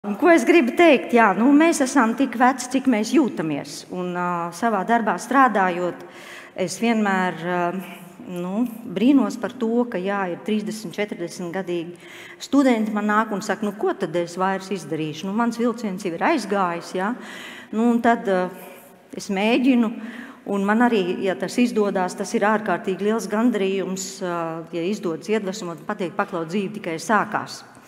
Forums LĪDERE 2021 | Rūta Lūse
Un ko es gribu teikt? Jā, nu mēs esam tik veci, cik mēs jūtamies, un, savā darbā strādājot, es vienmēr brīnos par to, ka, jā, ir 30, 40 gadīgi studenti man nāk un saka - nu ko tad es vairs izdarīšu, nu mans vilciens jau ir aizgājis, nu un tad es mēģinu un man arī, ja tas izdodas, tas ir ārkārtīgi liels gandarījums, ja izdodas iedvesmot, pateikt - paklau, dzīve tikai sākās.